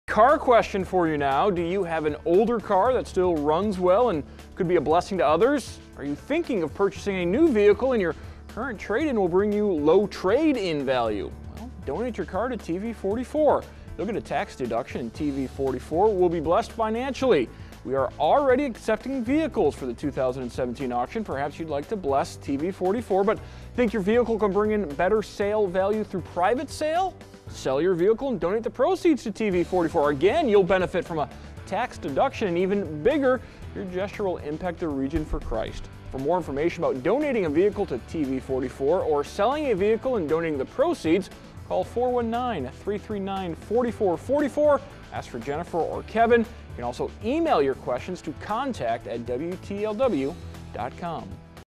Donate Your Car to TV-44
Here's a tax deductible opportunity that blesses the ministry of TV-44.